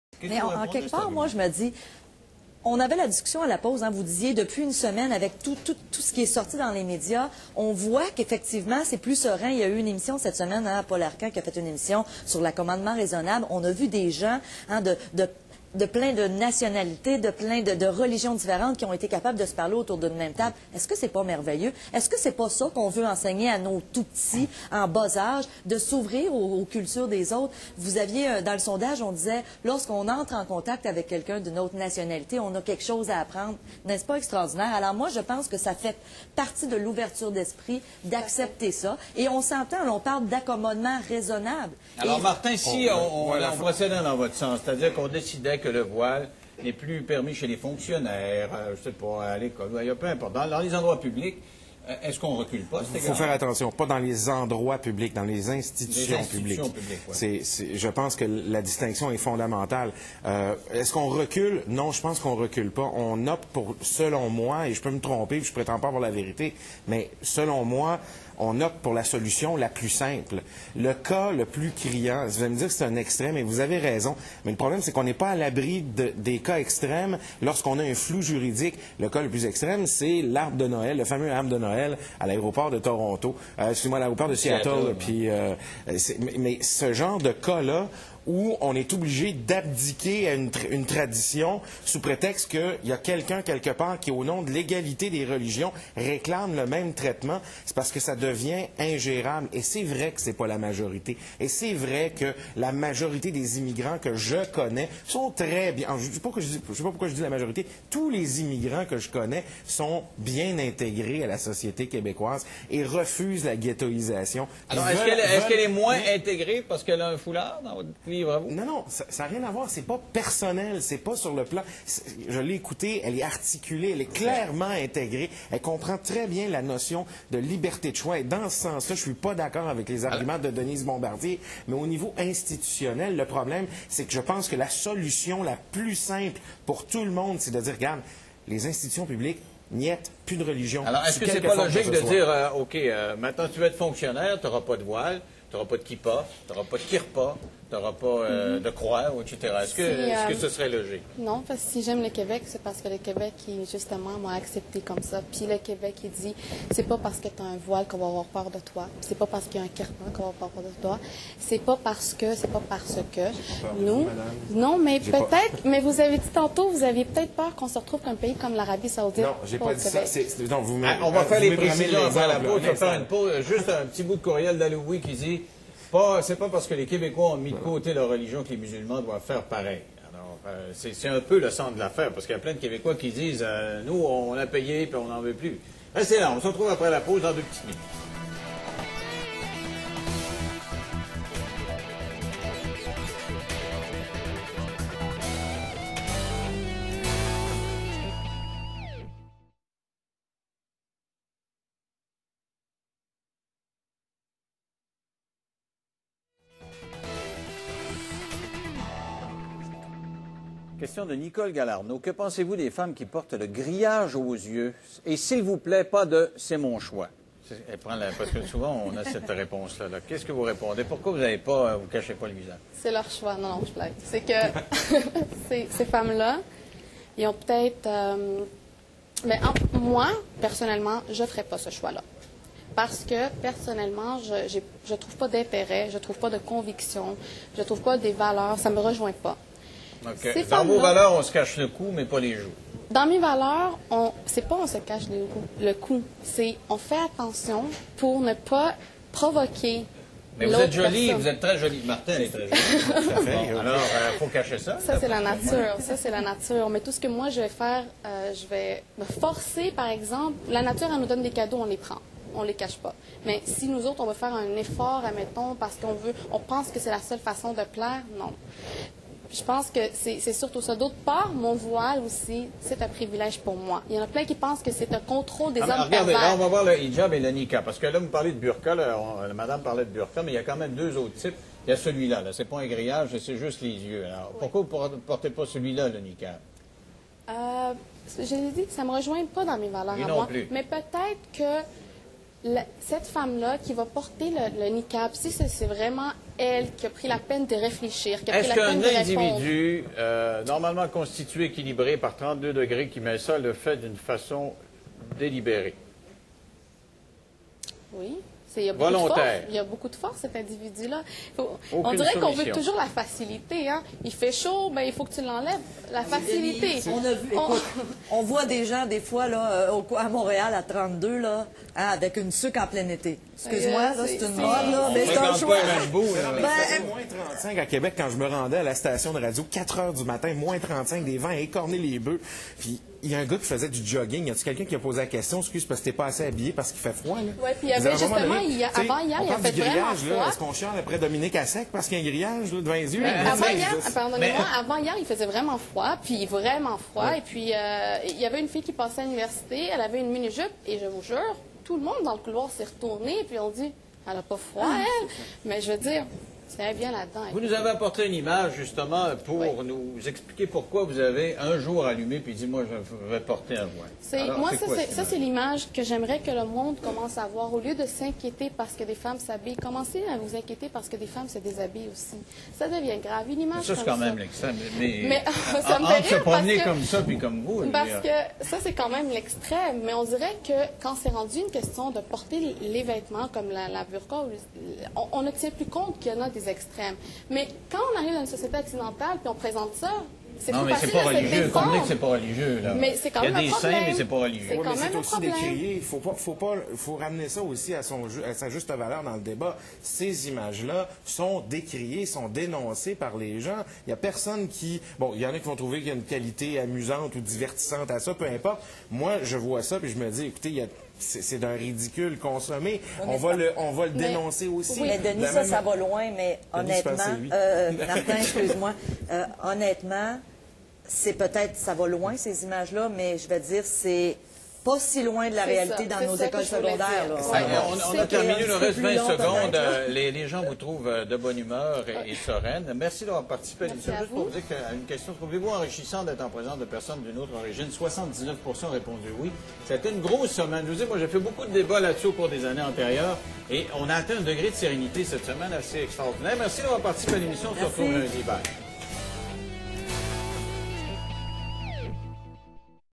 French interview about Muslim Woman PART 6
Discussing about hijab and islam In Quebec